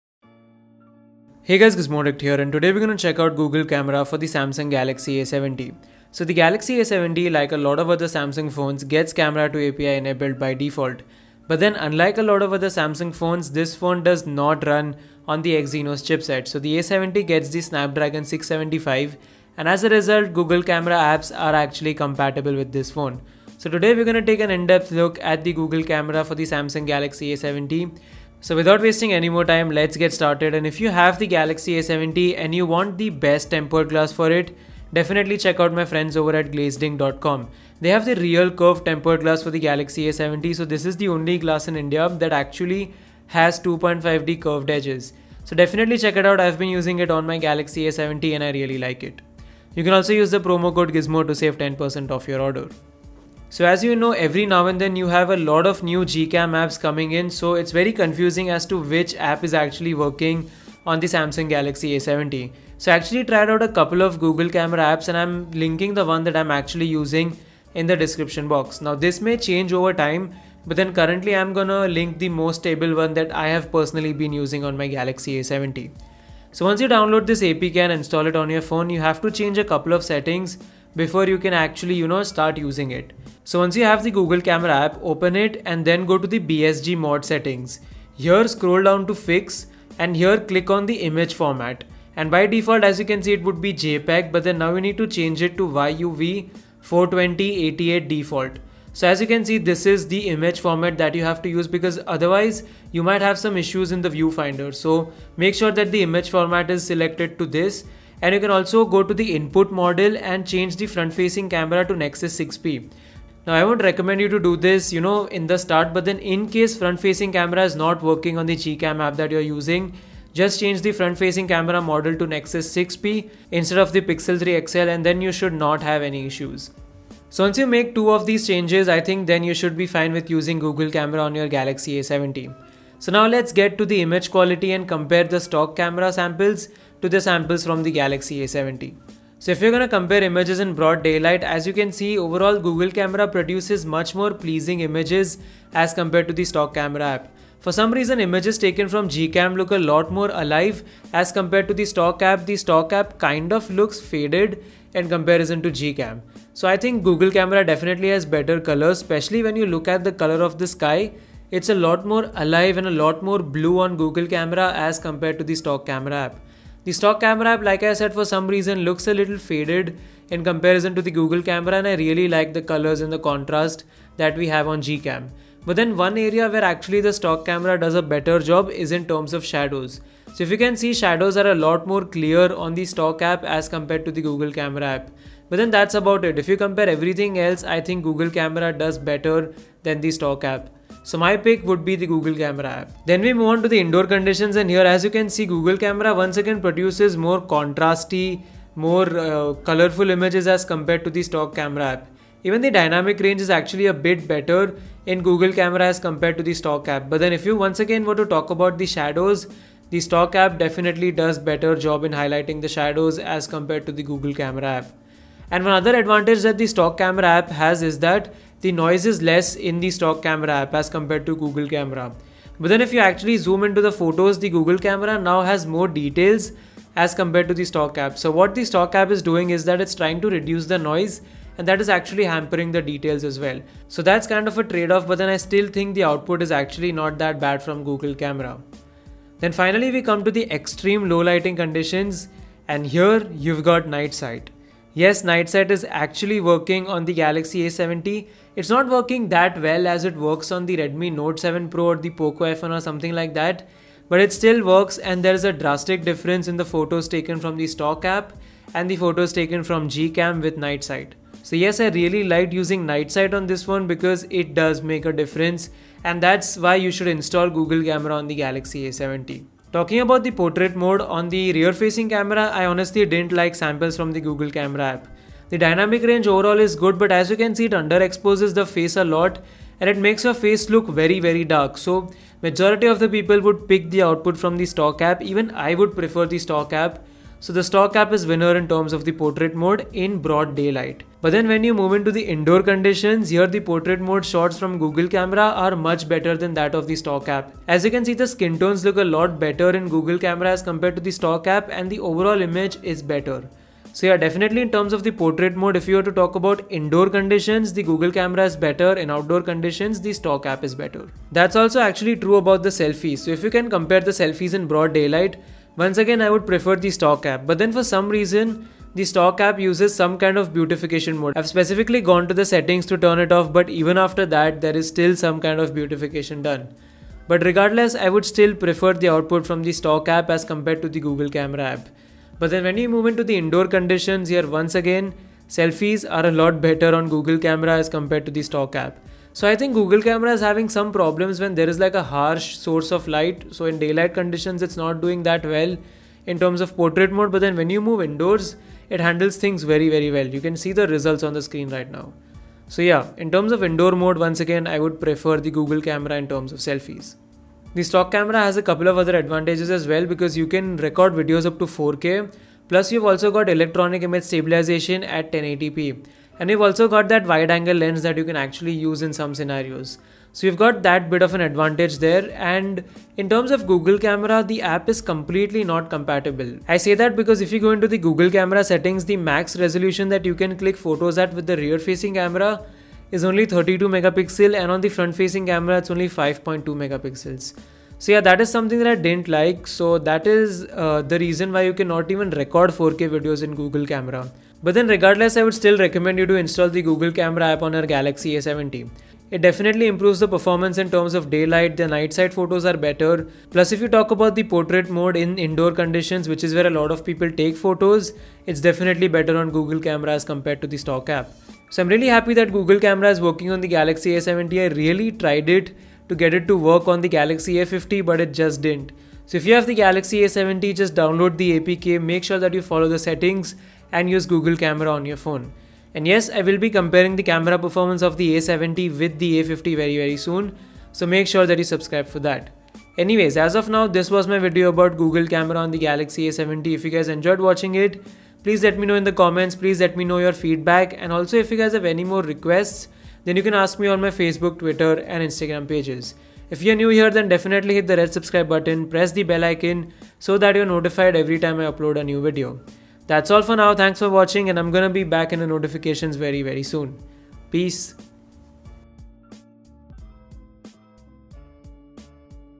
Google Camera On Samsung Galaxy A70 Review! Night Sight 😍😍
Samsung Galaxy A70 Google Camera vs Stock Camera. Which One Is Better?

This is a detailed comparison between the stock camera and google camera on the Samsung Galaxy A70. The A70 gets camera2api support and since it is based on Snapdragon 675, Google Camera actually works on it. So let's see how it compares with the stock app

Follow me on the following pages to get more Behind the Scenes content-